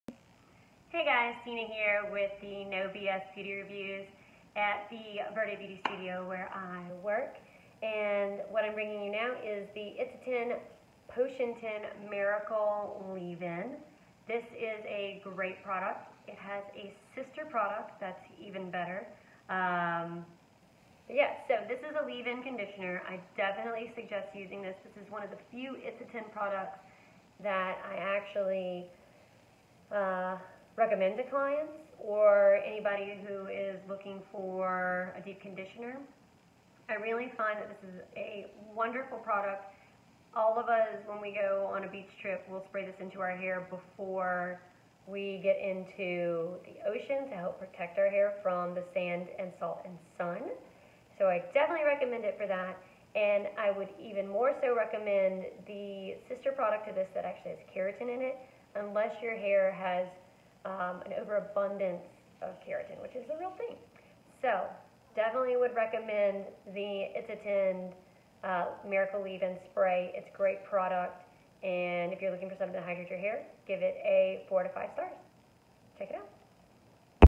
It's A 10 Miracle Leave-in Conditioner Review
This is a great product! I definitely suggest using it. It is one of the few It's A 10 products i recommend to clients looking for a deep conditioner. I find it's a wonderful product. We spray it into our hair at the beach before going into the ocean to protect it from the sand and salt. I more so recommend the sister product that has Keratin in it (unless your hair has an over abundance of Keratin). I give this product a 4 star review.

To see more product reviews or to get your own products to review, visit the following link to Influenster